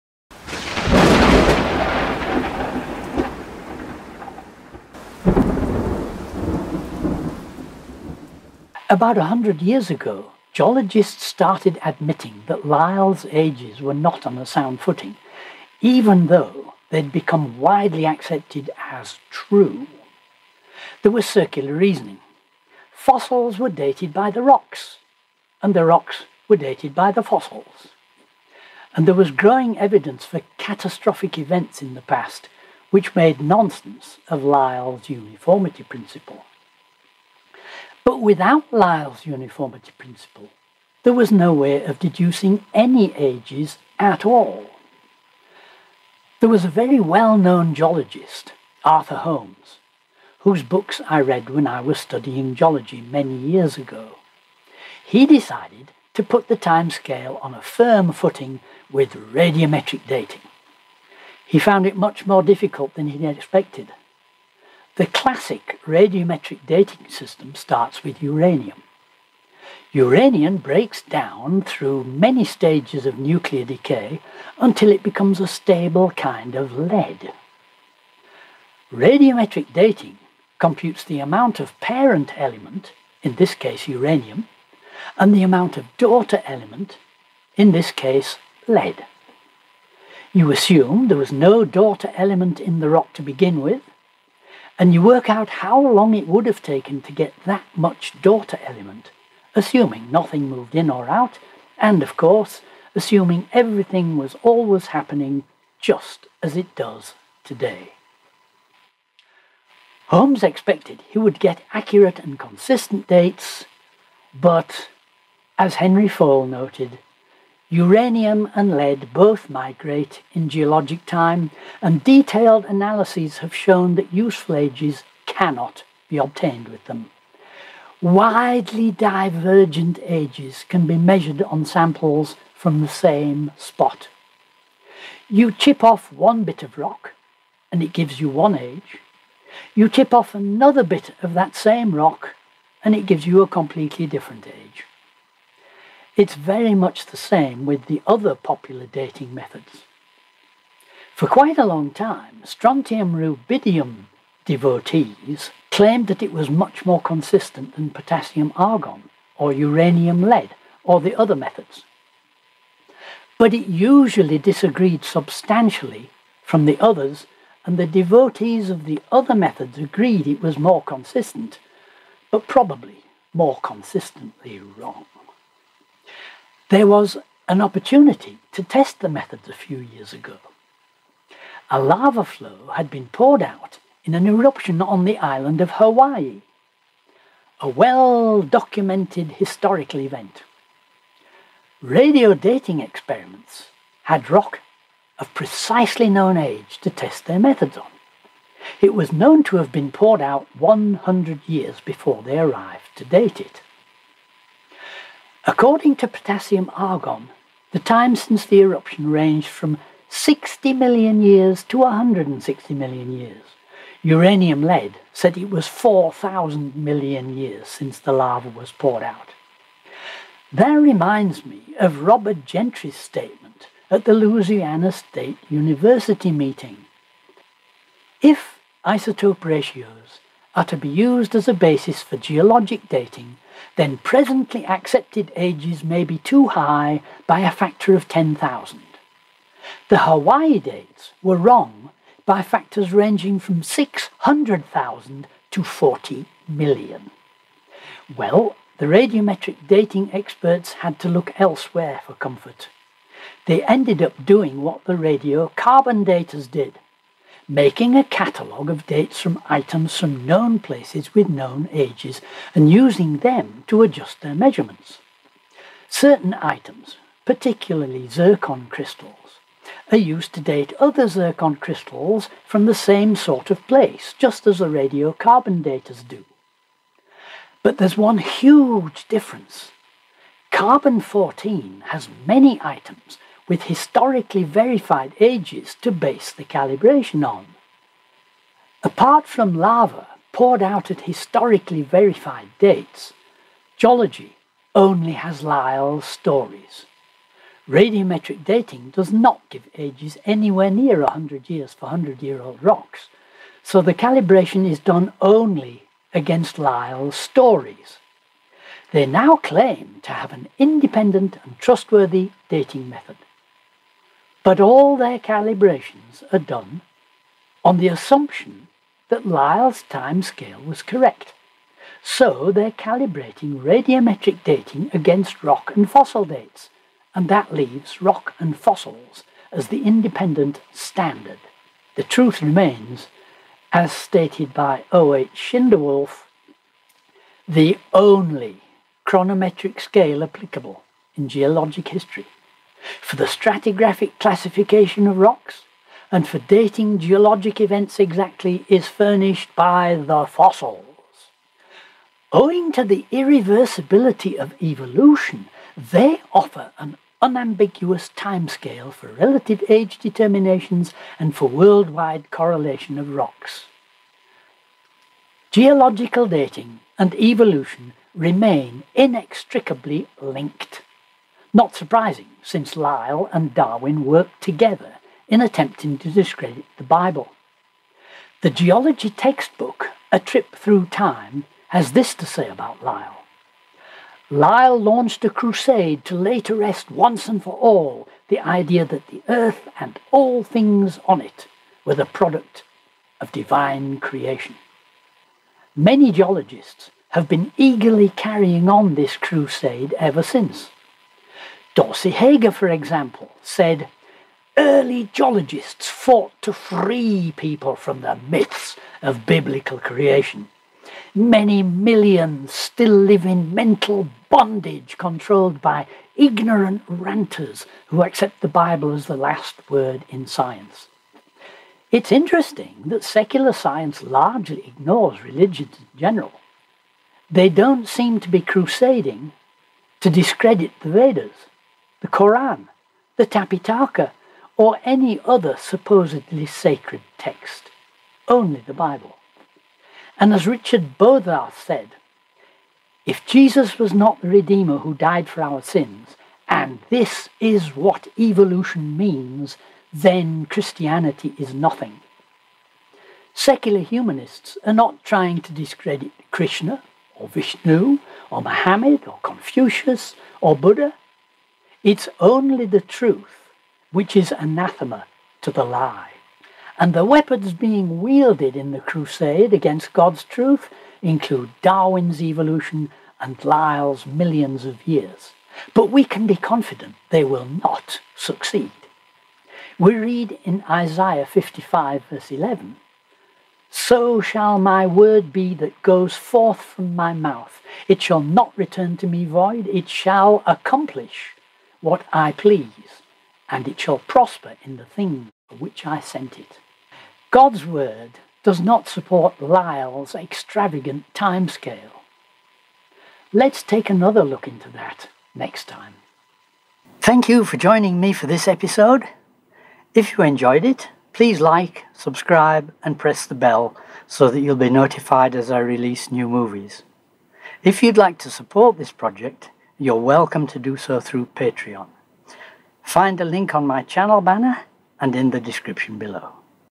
28 | Radiometric dating
The millions of years which Lyell produced by his uniformity principle are as flimsy as his lawyer-cunning arguments. We look at attempts to put the geological time scale on a more solid foundation using radiometric dating.